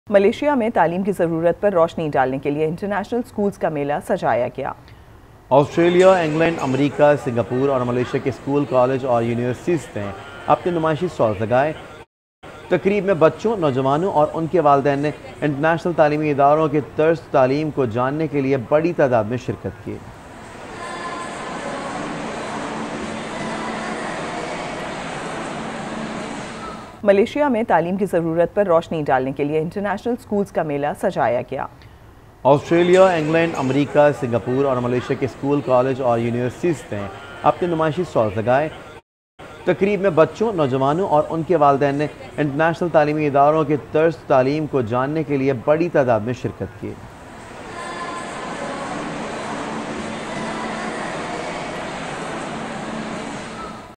International Schools Fair in Malaysia - Aaj News
International Schools Fair in Malaysia - 30 May 2023 - Aaj News

Spot Light With Munizae Jahangir:
shorturl.at/gwFU6